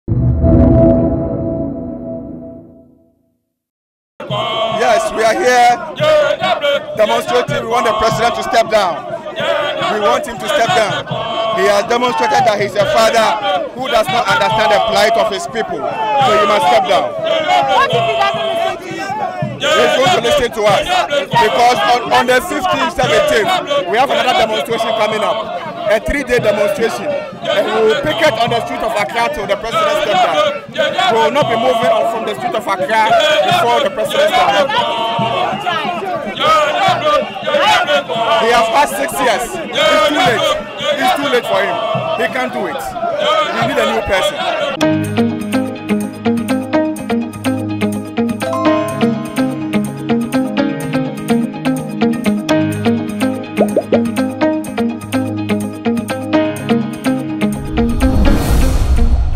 We have many demonstrations that will make Akufo-Addo step down - Mensah Thompson
Mensah Thompson asks the president to step down and more demonstrations will be held until he steps down.